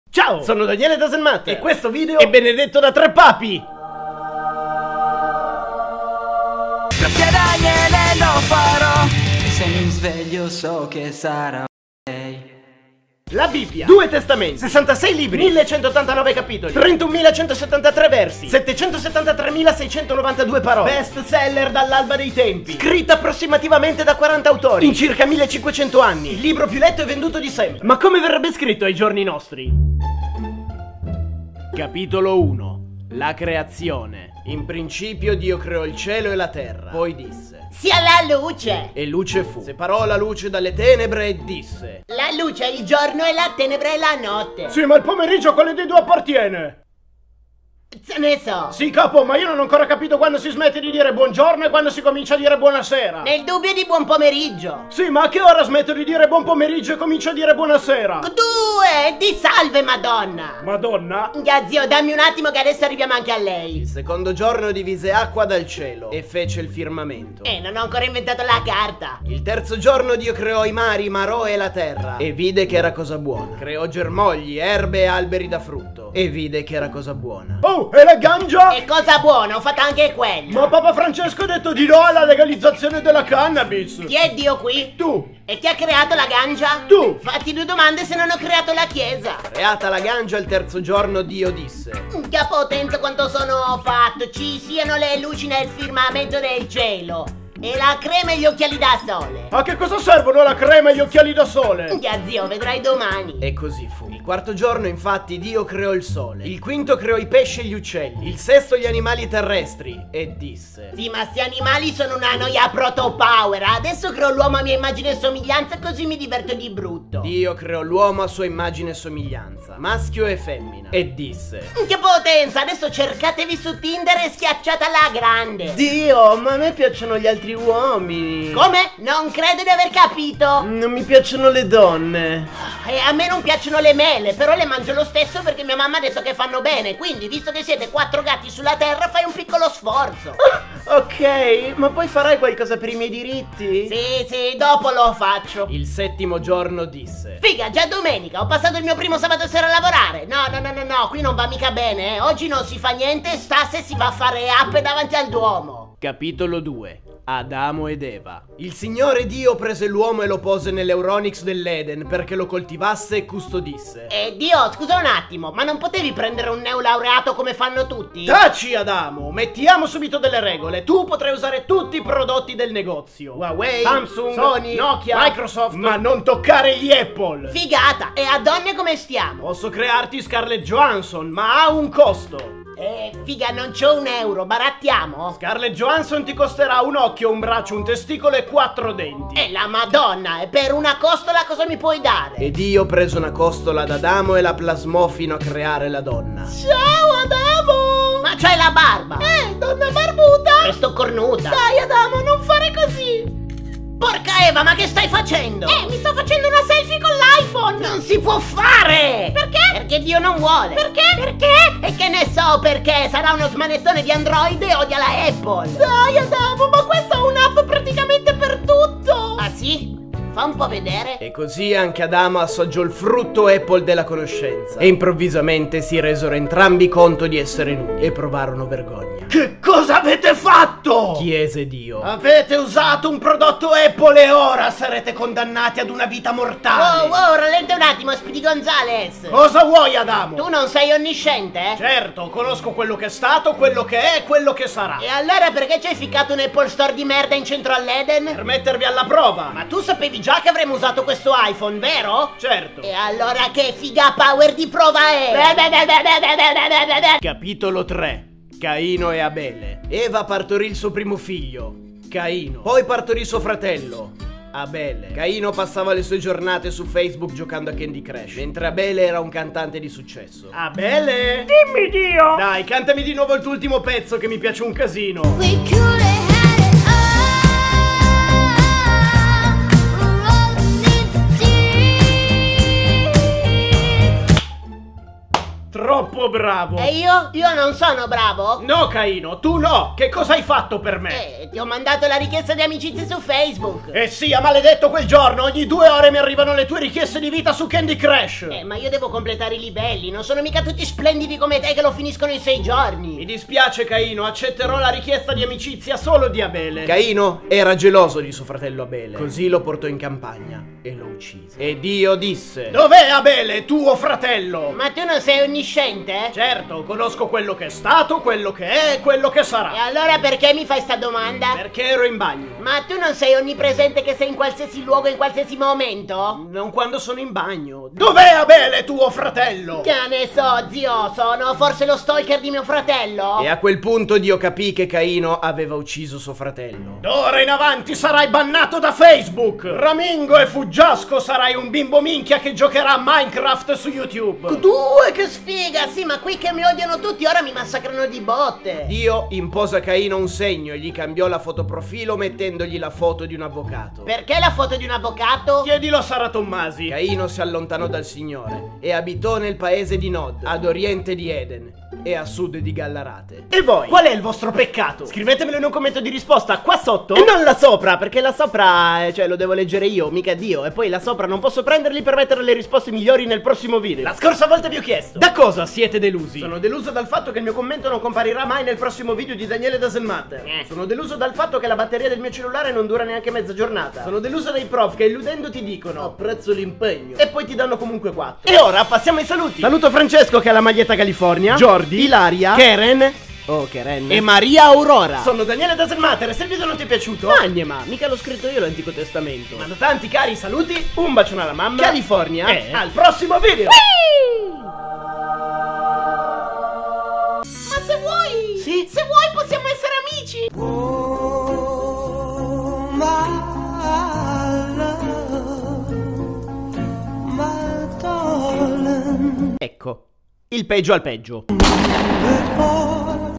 LA BIBBIA | PARTE 01 | AI GIORNI NOSTRI
Come potrebbe essere la Bibbia scritta ai giorni nostri? Ecco la mia risposta. PRIMA PARTE.
Ricordo a tutti che questo è un video comico e ironico che non mira a offendere la credenza o la non credenza di nessuno. Buon divertimento!